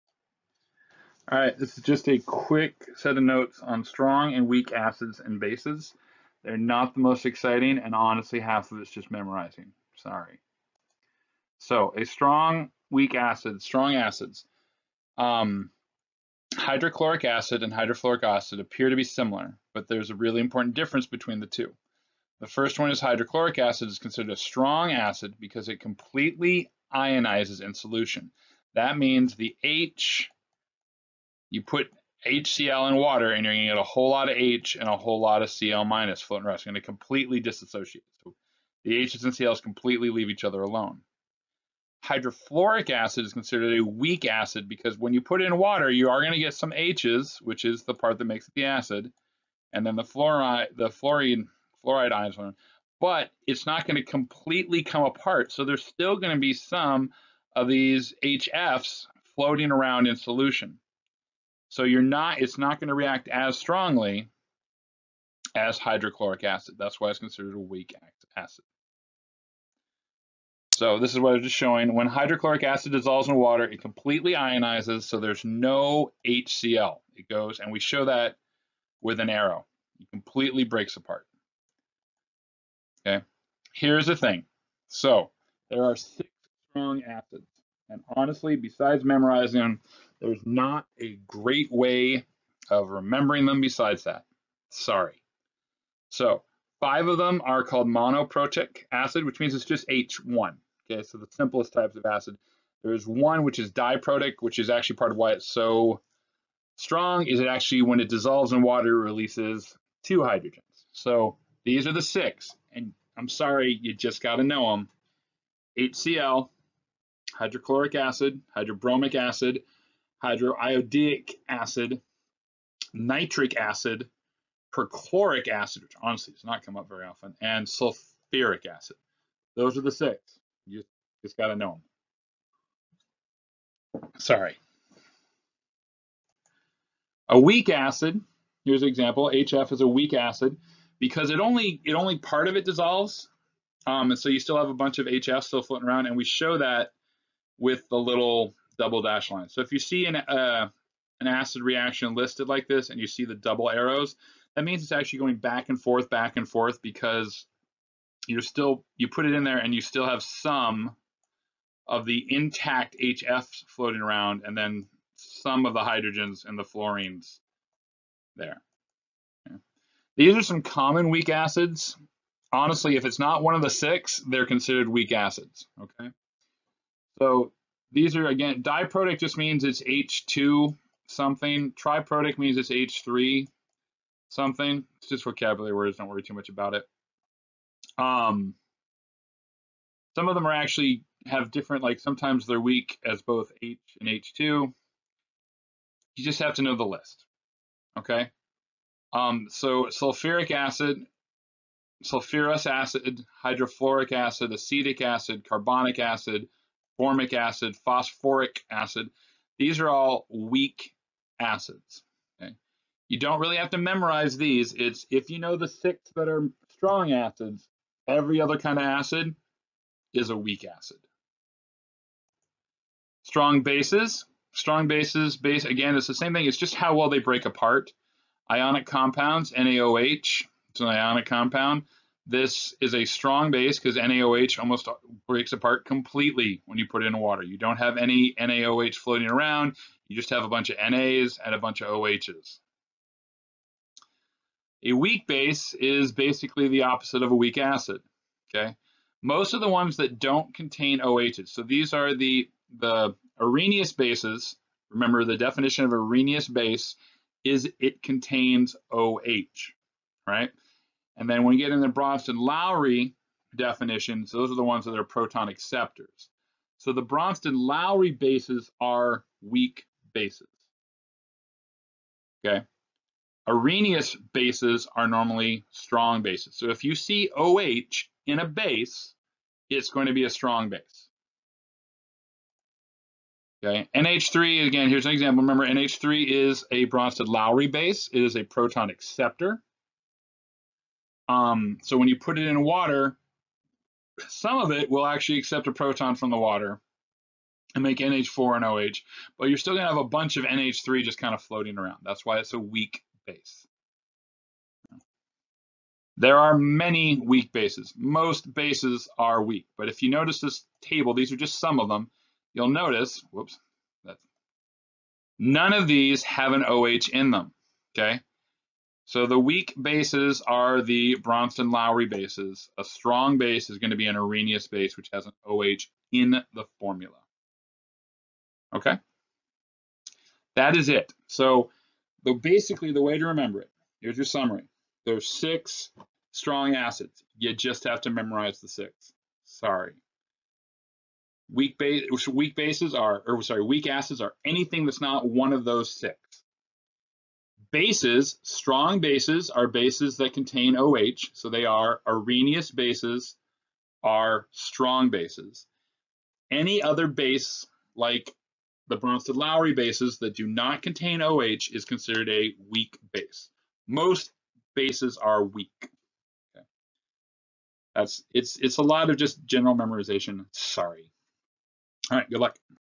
Strong and Weak Acids and Bases
Quick notes on Strong and Weak Acids and based based on Pearson textbook slides